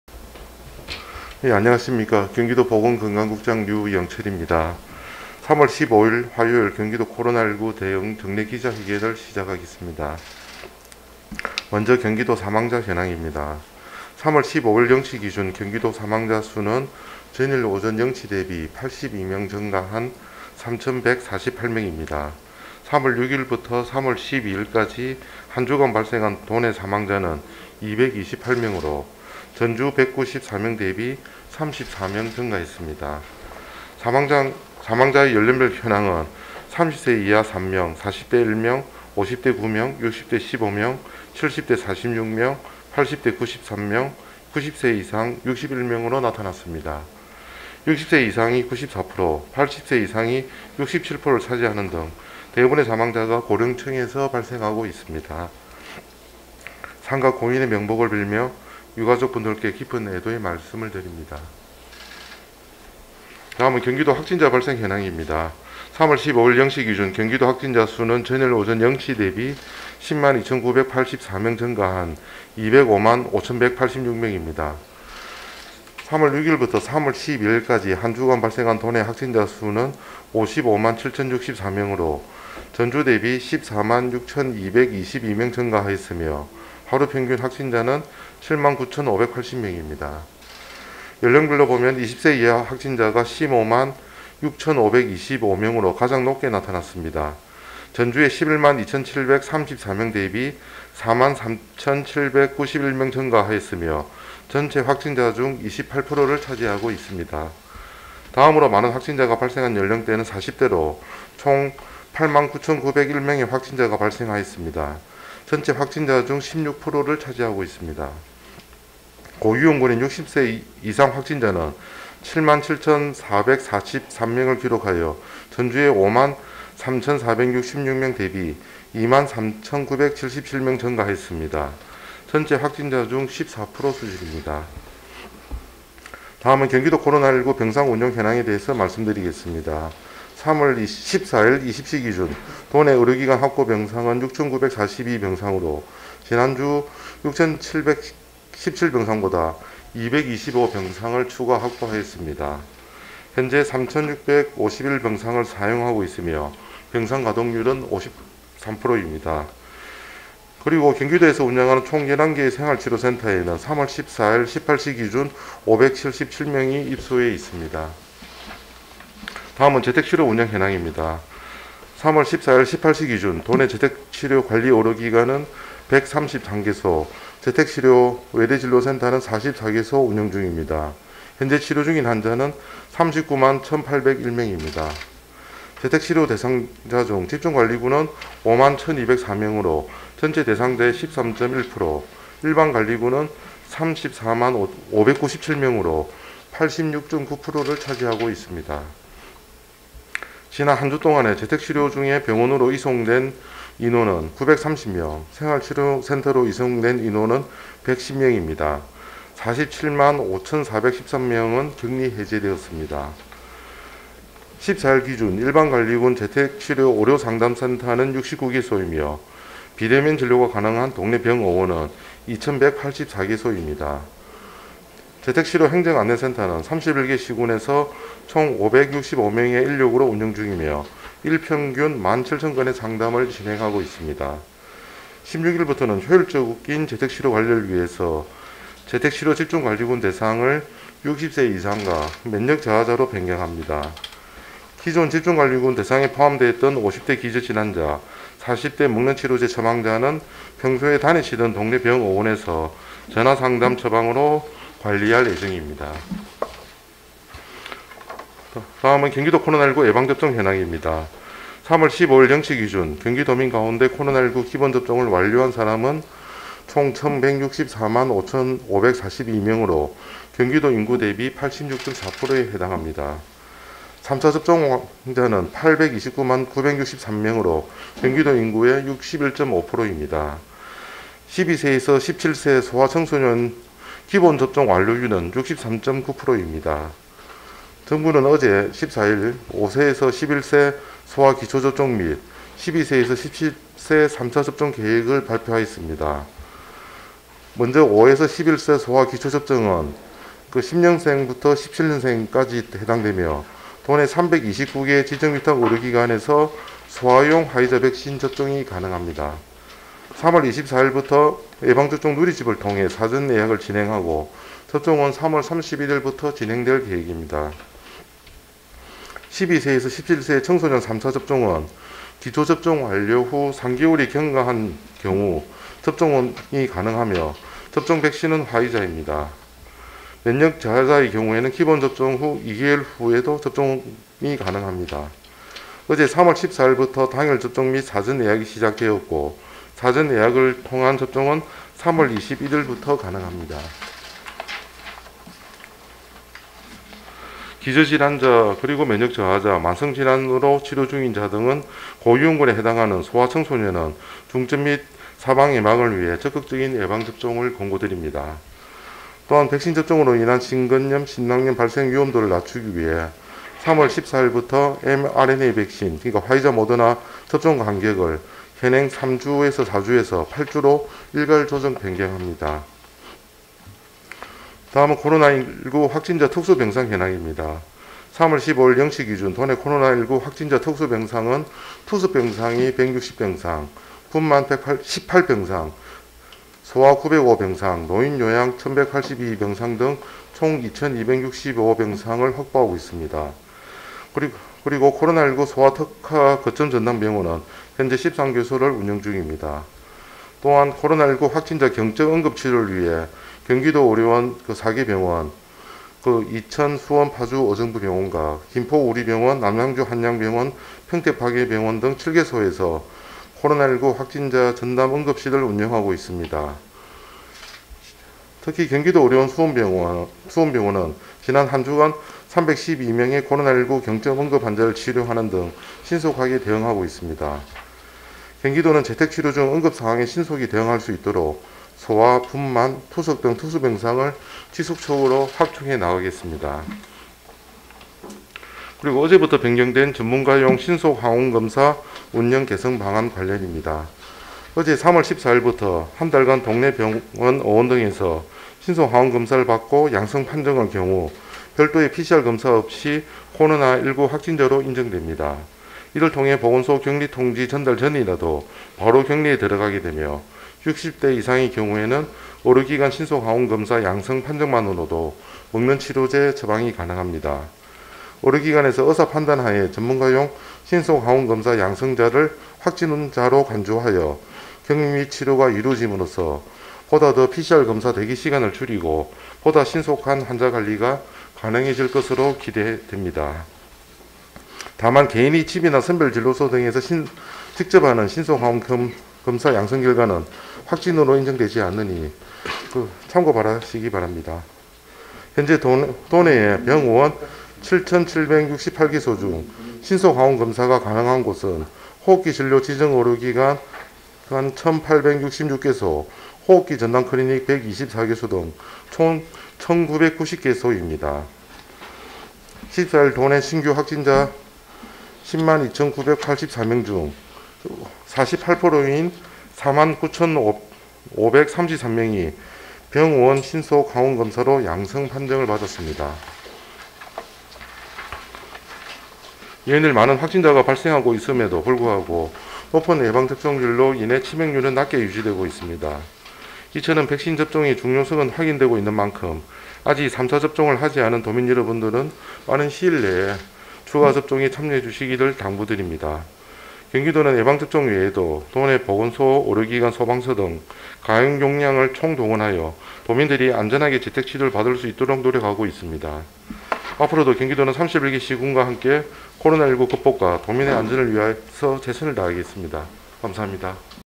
경기도 코로나19 대응 3월 15일 정례 기자회견
안녕하십니까? 경기도 보건건강국장 류영철입니다.

3월 15일 화요일 경기도 코로나19 대응 정례 기자회견을 시작하겠습니다.

□ 경기도 사망자 현황입니다.

◦3월 15일 0시 기준, 경기도 사망자 수는 전일 오전 0시 대비 82명 증가한 3,148명입니다.

◦3월 6일부터 3월 12일까지 한 주간 발생한 도내 사망자는 228명으로, 전주 194명 대비 34명 증가했습니다.

◦사망자 연령별 현황은 30세 이하 3명•40대 1명•50대 9명•60대 15명•70대 46명•80대 93명•90세 이상 61명으로 나타났습니다.

◦60세 이상이 94%, 80세 이상이 67%를 차지하는 등 대부분의 사망자가 고령층에서 발생하고 있습니다.

◦삼가 고인의 명복을 빌며, 유가족분들께 깊은 애도의 말씀을 드립니다.

□ 다음은 경기도 확진자 발생 현황입니다.

◦3월 15일 0시 기준, 경기도 확진자 수는 전일 오전 0시 대비 10만 2,984명 증가한 205만 5,186명입니다.

◦3월 6일부터 3월 12일까지 한 주간 발생한 도내 확진자 수는 55만 7,064명으로 전주 대비 14만 6,222명 증가하였으며, 하루 평균 확진자는 7만 9,580명입니다.

◦연령별로는 20세 이하 확진자가 15만 6,525명으로 가장 높게 나타났습니다.
  전주 11만 2,734명 대비 4만 3,791명 증가하였으며, 전체 확진자 중 28%를 차지하고 있습니다.

◦다음으로 많은 확진자가 발생한 연령대는 40대로 총 8만 9,901명의 확진자가 발생하였으며, 전체 확진자 중 16%를 차지하고 있습니다.

◦고위험군인 60세 이상 확진자는 7만 7,443명을 기록하여 전주 5만 3,466명 대비 2만 3,977명 증가하였습니다. 전체 확진자 중 14% 수준입니다.

□ 경기도 코로나19 병상운용 현황에 대해 말씀드리겠습니다.

◦3월 14일 20시 기준, 도내 의료기관 확보 병상은 6,942병상으로 지난주 6,717병상보다 225병상을 추가 확보하였습니다.

◦현재 3,651병상을 사용하고 있으며 병상 가동률은 53%입니다.

◦이외에도 경기도에서 운영하는 총 11개의 생활치료센터에는 3월 14일 18시 기준 577명이 입소해 있습니다.

□ 다음은 재택치료 운영현황입니다.

◦3월 14일 18시 기준, 도내 재택치료 관리 의료기관은 133개소, 재택치료자 외래진료센터는 44개소 운영 중입니다. 현재 치료 중인 환자는 39만 1,801명입니다.

◦재택치료 대상자 중 집중관리군은 5만 1,204명으로 전체 대상자의 13.1%, 일반관리군은 34만 597명으로 86.9%를 차지하고 있습니다.

◦지난 한 주 동안 재택치료 중 병원으로 이송된 인원은 930명, 생활치료센터로 이송된 인원은 110명입니다. 47만 5,413명은 격리해제 되었습니다.

◦14일 기준 일반관리군 재택치료 의료상담센터는 69개소이며, 비대면 진료가 가능한 동네 병의원은 2,149개소입니다.

◦재택치료 행정안내센터는 31개 시군에서 총 565명의 인력으로 운영 중이며, 일평균 1만 7천건의 상담을 진행하고 있습니다.

◦16일부터 재택치료 집중관리군 기준이 60세 이상, 면역저하자로 변경됩니다.

◦기존 집중관리군 대상에 포함되었던 50대 기저질환자, 40대 먹는 치료제 처방자는 다니던 동네병의원에서 전화상담•처방으로 관리할 예정입니다.

□ 다음은 경기도 코로나19 예방접종 현황입니다.

◦3월 15일 0시 기준, 경기도민 가운데 코로나19 기본접종을 완료한 사람은 총 1,164만 5,542명으로 경기도 인구 대비 86.4%에 해당합니다.

◦3차 접종자는 829만 963명으로, 경기도 인구 대비 61.5%입니다.

◦12~17세 소아•청소년 1차 접종률은 66.9%이며, 기본접종 완료율은 63.9%입니다.

◦정부는 어제(14일) 5~11세 소아 기초접종 및 12~17세 3차접종 계획을 발표하였습니다.

◦먼저 5~11세 소아 기초접종은 `10년생(생일 미도과자)부터 `17년생(생일 도과자)까지가 해당되며, 소아용 화이자 백신을 통해 도내 329개 지정 위탁의료기관에서 접종 가능합니다.

◦3월 24일부터 예방접종 누리집을 통해 사전예약을 진행하고, 접종은 3월 31일부터 개시할 예정입니다.

◦12~17세 청소년 3차접종은 기초접종 완료 후 3개월이 경과한 경우 접종가능하며, 접종백신은 화이자입니다.

◦면역저하자의 경우에는 기본접종 후 2개월 후 접종도 가능합니다.

◦어제 3월 14일부터 당일접종 및 사전예약이 시작되었고, 사전예약을 통한 접종은 3월 21일부터 가능합니다.

◦기저질환자•면역저하자•만성질환으로 치료 중인 자 등 고위험군에 해당하는 소아•청소년은 중증 및 사망 예방을 위해 적극적인 예방접종을 권고드립니다.

◦또한, 백신접종으로 인한 심근염•심낭염 발생 위험도를 낮추기 위해 3월 14일부터 mRAN백신(화이자, 모더나) 접종간격이 현행 3~4주에서 8주로 일괄 조정합니다.

□ 다음은 코로나19 확진자 특수병상 현황입니다.

◦3월 15일 0시 기준, 도내 코로나19 확진자 특수병상은 투석 160병상, 분만 18병상, 소아 905병상, 노인요양 1,182병상 등 총 2,265병상을 확보하고 있습니다.

◦코로나19 소아특화 거점전담병원은 현재 13개소를 운영 중입니다.

◦또한, 코로나19 확진자 경증 응급 치료를 위해 경기도의료원 4개 병원(이천, 수원, 파주 의정부), 김포우리병원, 남양주 한양병원, 평택 박애병원 7개소에서 ‘코로나19 확진자 전담 응급실’을 운영하고 있습니다.

◦경기도의료원 수원병원은 지난 한 주간 312명의 코로나19 경증 응급 환자를 치료하였습니다.

◦경기도는 재택치료 중 응급상황에 신속히 대응할 수 있도록 소아, 분만, 투석 등 특수병상을 지속적으로 확충해 나가겠습니다.

□ 전문가용 신속항원검사 운영 개선방안 관련입니다.

◦3월 14일부터 한 달간 동네 병원, 의원 등에서 신속항원검사를 받고 ‘양성’ 판정을 받은 경우, 별도 PCR 검사 없이 코로나19 확진자로 인정됩니다.

◦이를 통해 보건소 격리 통지 전달 전이라도 바로 격리에 들어가게 되며, 60대 이상의 경우에는 의료기관 신속항원검사 양성 판정만으로 먹는 치료제 처방이 가능합니다.

◦의료기관에서 의사 판단 하에 전문가용 신속항원검사 양성자를 확진으로 간주하여 격리 및 치료가 이뤄짐으로써 보다 PCR 검사 대기 시간을 줄이고 보다 신속한 환자 관리가 가능해질 것으로 기대됩니다.

◦다만, 개인이 집이나 선별진료소 등에서 직접 하는 신속항원검사 양성 결과는 확진으로 인정되지 않습니다.

◦현재 도내 병의원 7,768개소 중 신속항원검사가 가능한 곳은 호흡기 진료 지정 의료기관 1,866개소, 호흡기전담클리닉 124개소 등 1,990개소입니다.
  (심평원 사이트 기준)

◦14일 도내 신규 확진자 10만 2,984명 중 48%인 4만 9,533명이 병의원 신속항원검사로 양성 판정을 받았습니다.

◦연일 많은 확진자가 발생하고 있음에도 불구하고, 높은 예방접종률로 인해 치명률은 낮게 유지되고 있습니다.

◦이처럼 백신접종의 중요성은 확인되고 있는 만큼, 아직 3차 접종을 하지 않은 도민 여러분들은 빠른 시일 내에 추가접종에 참여해주시기를 당부드립니다.

◦또한, 경기도는 예방접종 이외에도 도내 보건소-의료기관-소방서 등 가용역량을 총동원하여 도민들이 안전하게 재택치료를 받으실 수 있도록 노력하고 있습니다.

◦앞으로도 경기도는 31개 시군과 함께 코로나19 극복과 도민의 안전을 위해 최선을 다하겠습니다. 감사합니다.